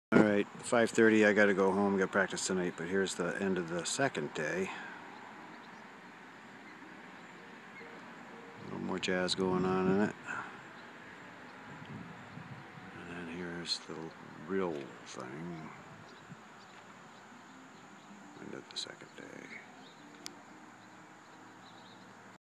2nd_day_at_hamburg_going_home_for_practice_IMG_1308.MOV
The end of the second day painting at Hamburg Cove. Going home early for rehearsal with my Band the Side Doors. for more Cove paintings.